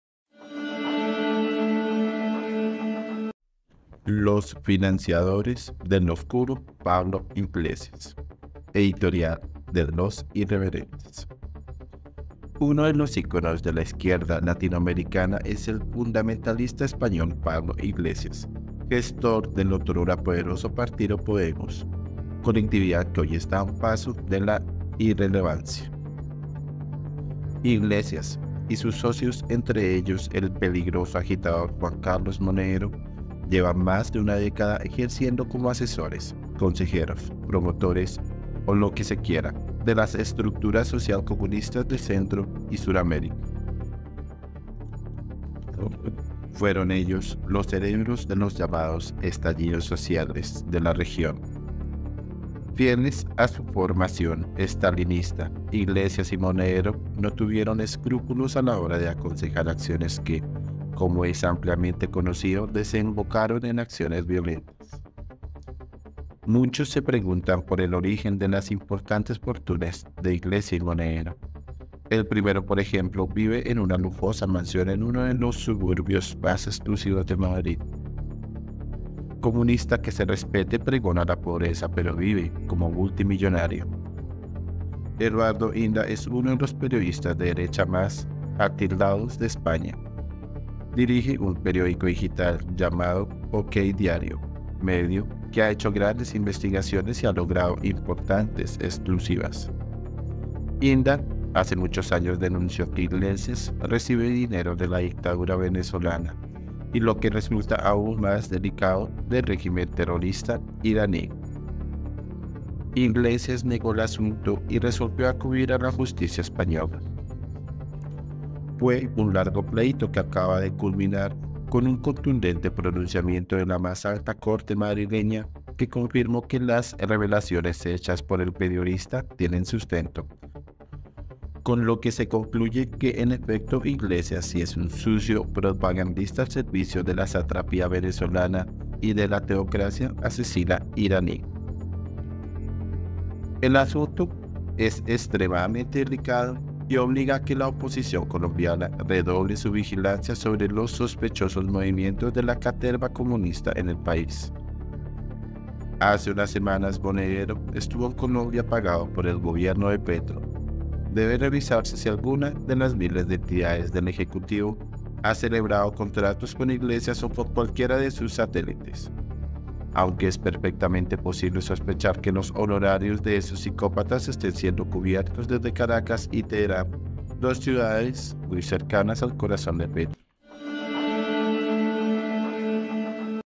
LOS FINANCIADORES DEL OSCURO PABLO IGLESIAS ⛔ Editorial Los Irreverentes
Iglesias y sus socios, entre ellos el peligroso agitador Juan Carlos Monedero llevan más de una década ejerciendo como asesores, consejeros, promotores o lo que se quiera, de las estructuras socialcomunistas de centro y sur América.